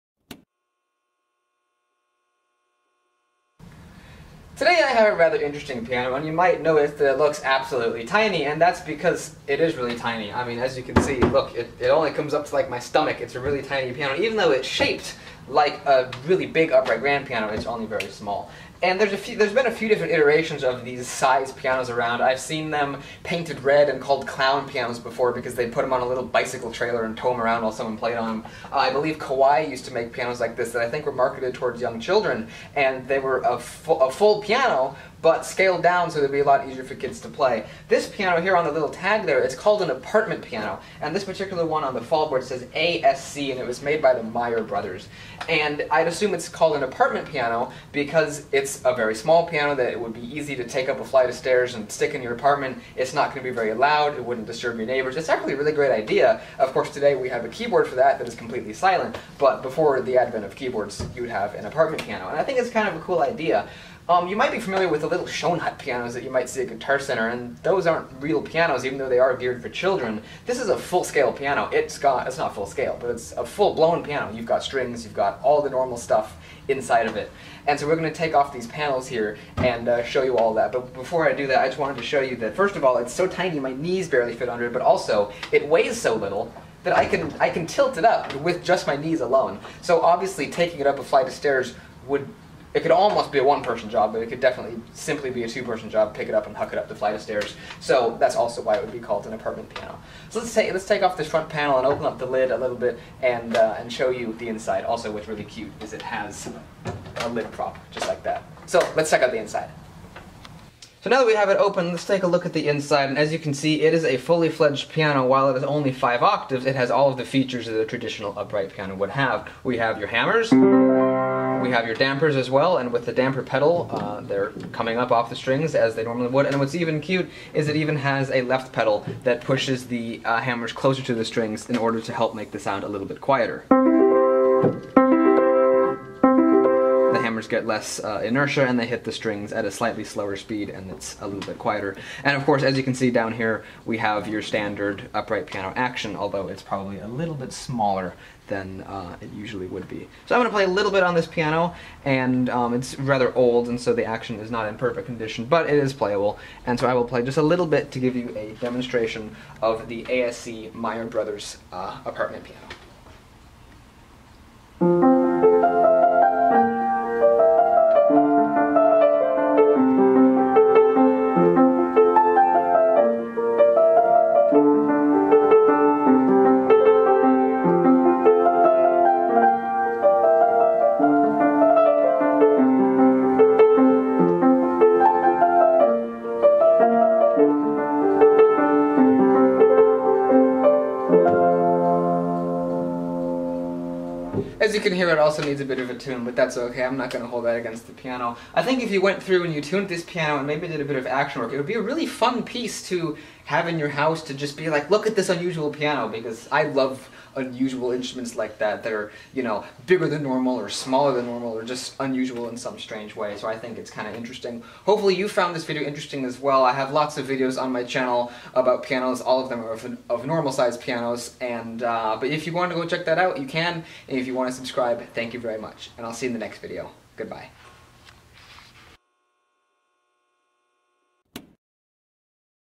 A S C Apartment Piano Mayer Brothers
This is an odd little piano I ran across, and I found it interesting that it was a real, fully functioning piano, but in a miniature form. Whether it was made for children or small apartments, it's cool either way.

Here's where I found this piano:
Peninsula Piano Brokers
4333 El Camino Real, Palo Alto, CA
94306